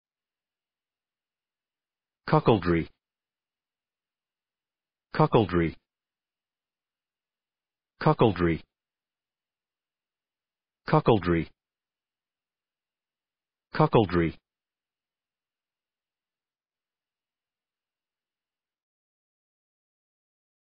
How to pronounce cuckoldry
cuckoldry American English pronunciation. How to pronounce cuckoldry correctly. How to say cuckoldry in proper American English.